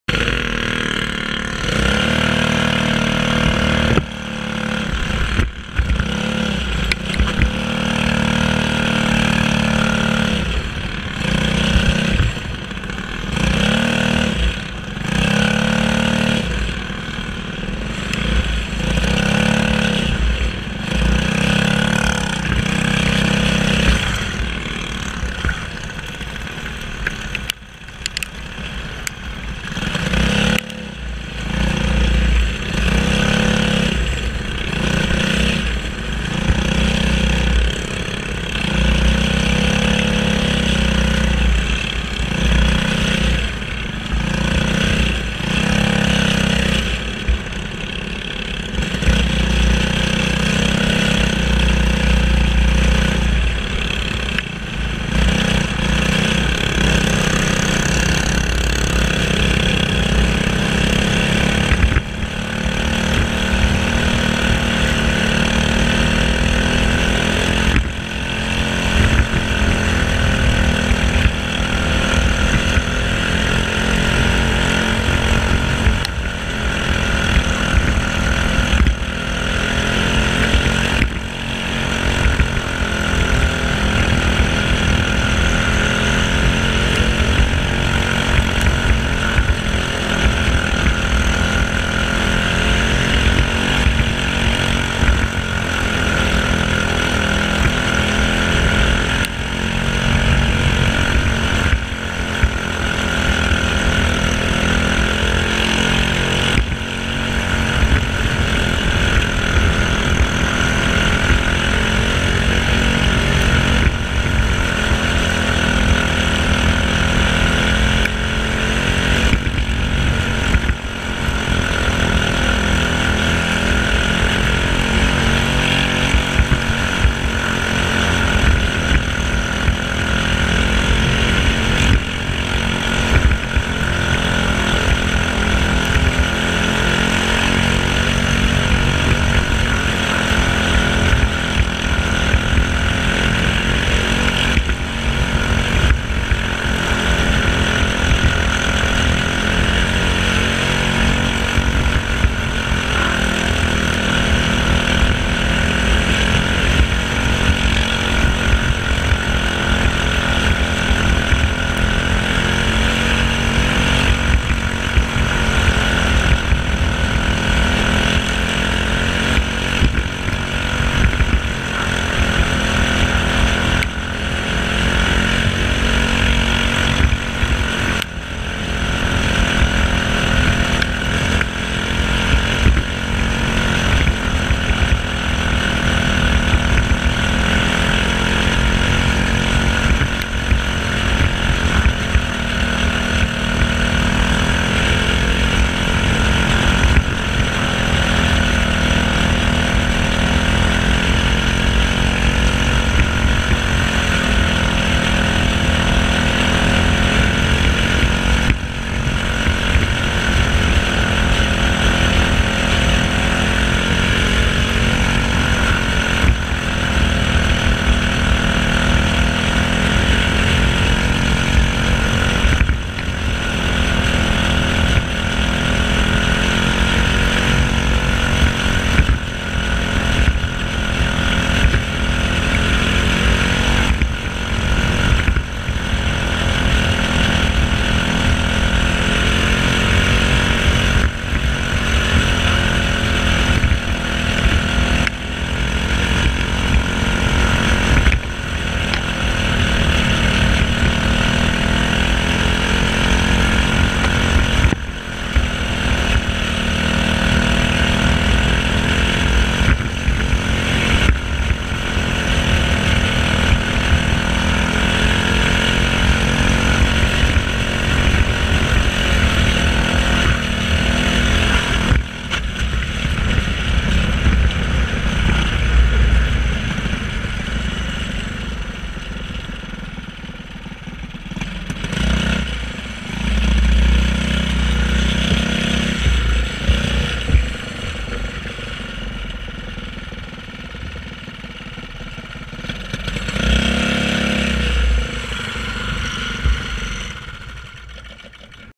Poughkeepsie Raceway Sr. 206 4/2/17
Today was a practice day. We struggled, but we learned a few things. Leave a comment what you think of the new chincam!